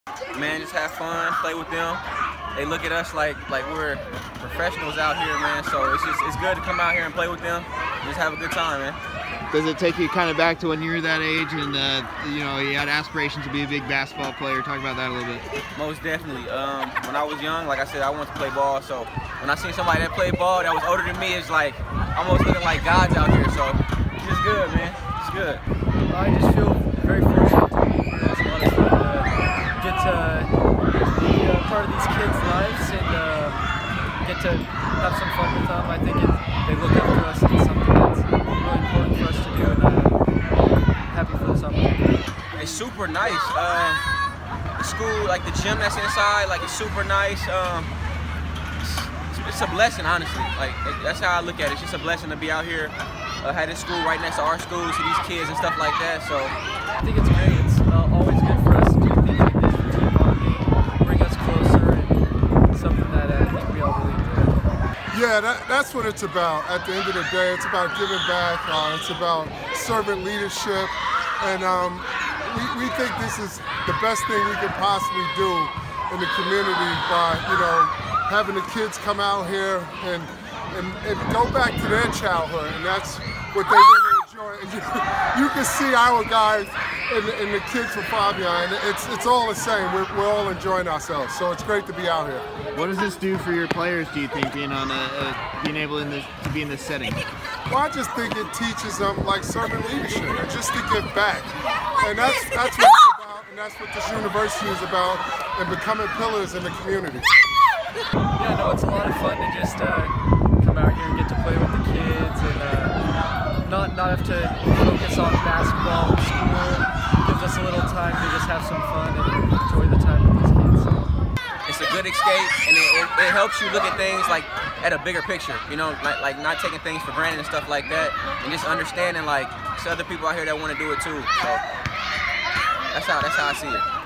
CU Men's Basketball Faubion School Visit - 12/17/2019
The Concordia University men's basketball team visited Faubion School to play with students during recess on Dec. 17, 2019.